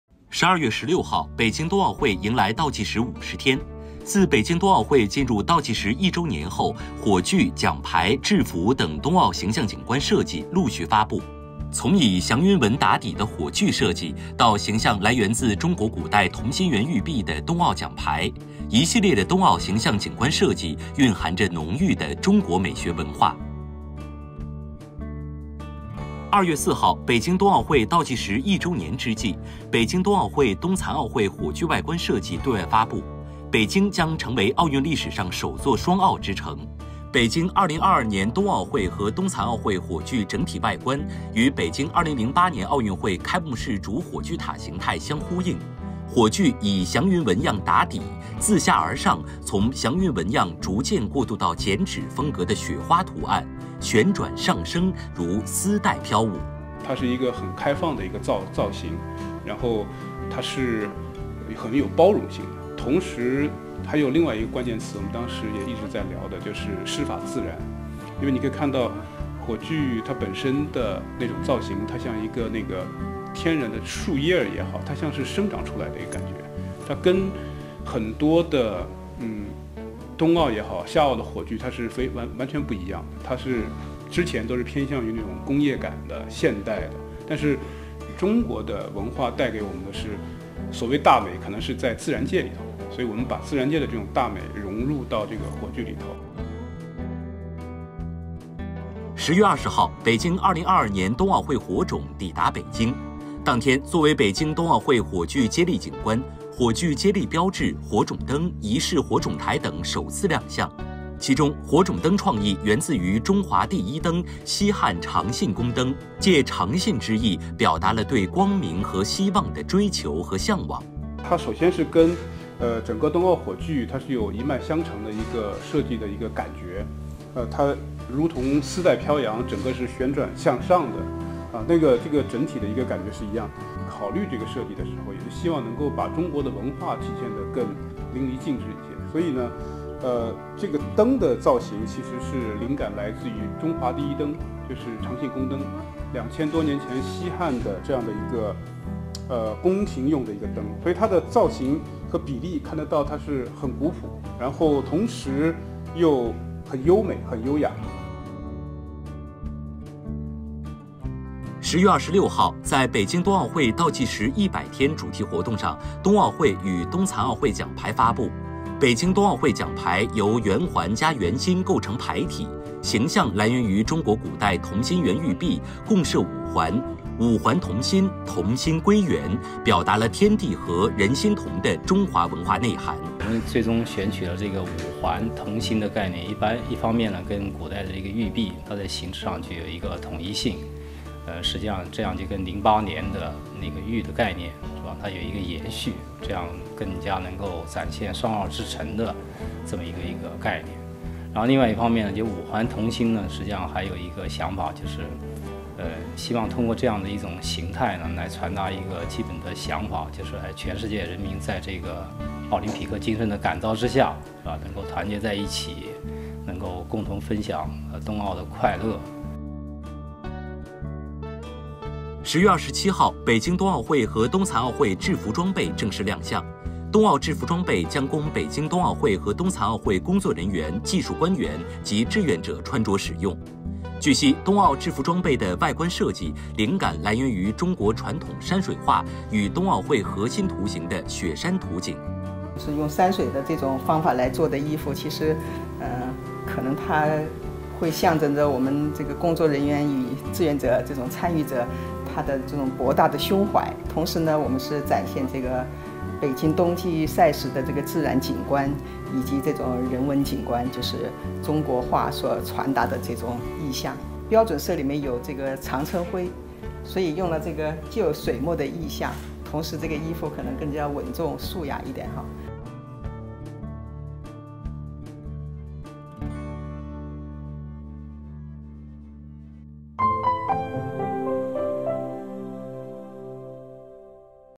冬奥倒计时50天 看火炬、奖牌、制服蕴含的中国美学
12月16日，北京冬奥会迎来倒计时50天。自北京冬奥会进入倒计时一周年后，火炬、奖牌、制服等冬奥形象景观设计陆续发布。

从以祥云纹“打底”的火炬设计，到形象来源自中国古代同心圆玉璧的冬奥奖牌，一系列的冬奥形象景观设计蕴含着浓郁的“中国美学”文化。